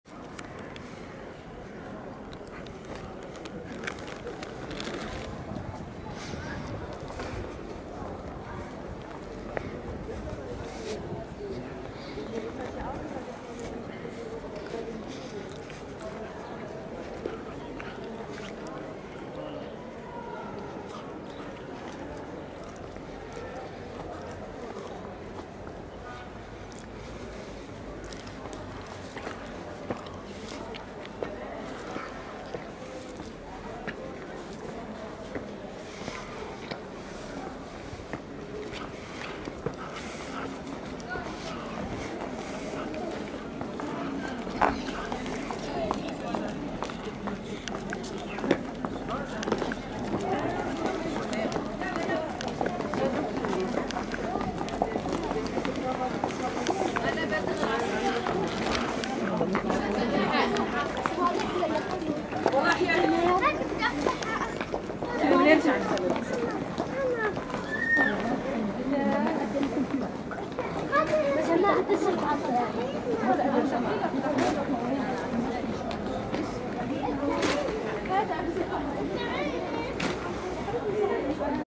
Al-Aqsa Mosque in old Jerusalem
Al-Aqsa Mosque is the third holiest site in Islam and is located in the Old City of Jerusalem. The site on which the silver domed mosque sits, along with the Dome of the Rock is the Temple Mount, the holiest site in Judaism, the place where the Temple is generally accepted to have stood. Muslims believe that Muhammad was transported from the Sacred Mosque in Mecca to al-Aqsa during the Night Journey. Islamic tradition holds that Muhammad led prayers towards this site until the seventeenth month after the emigration, when God directed him to turn towards the Ka'aba.